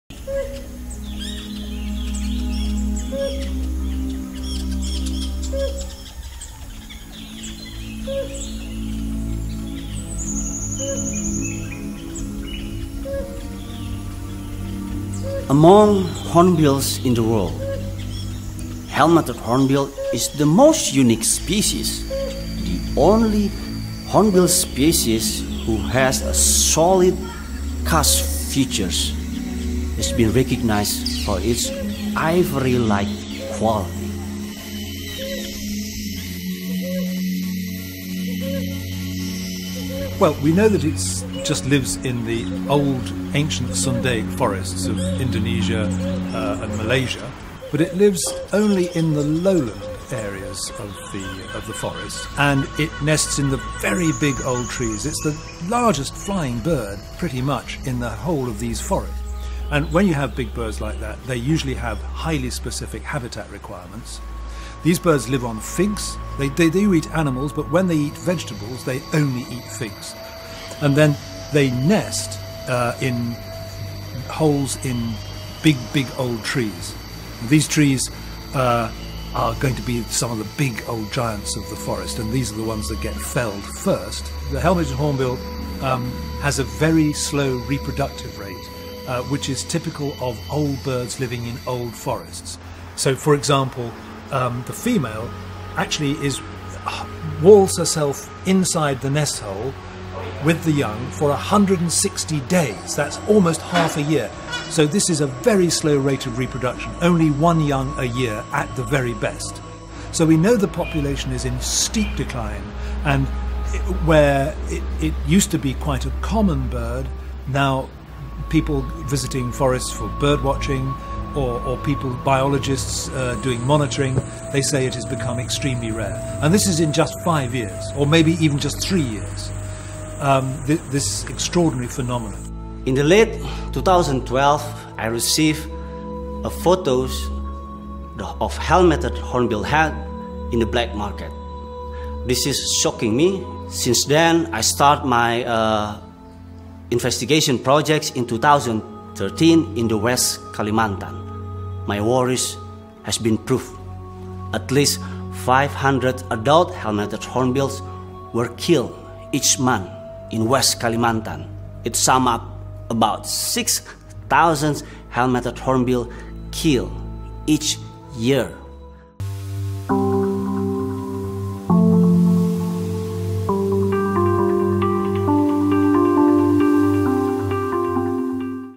El CALAO de YELMO. Animal muy raro en PELIGRO de EXTINCIÓN | National Geographic en Español
Es la única especie de bucerótido con unas facciones sólidas, como un casco. Ha sido reconocido por sus cualidades similares al marfil.